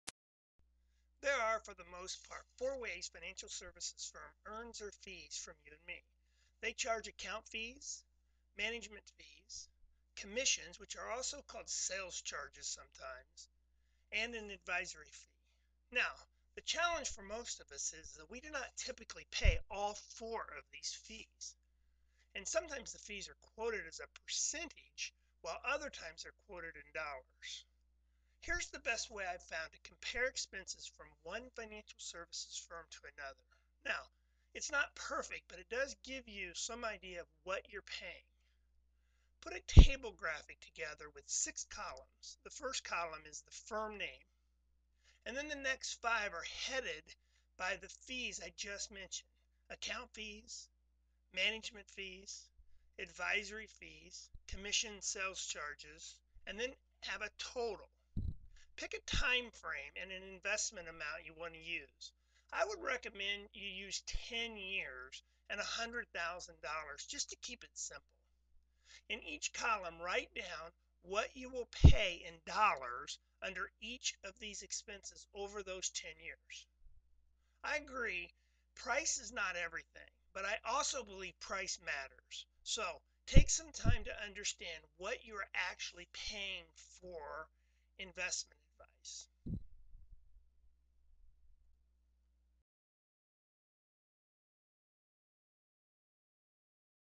Explaining Financial Services Fees
What we pay for financial advice and serivces can be very confusing.